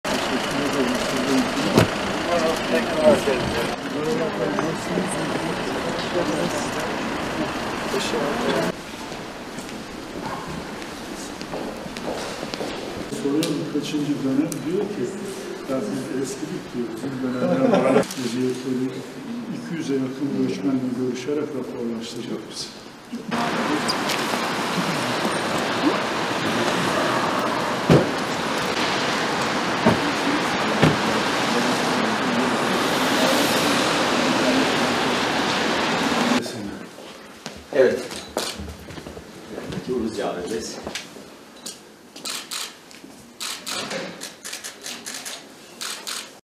Kamu Başdenetçisi Şeref Malkoç, Elazığ'da
Kamu Başdenetçisi Şeref Malkoç, Elazığ'da ziyaretler gerçekleştirdi.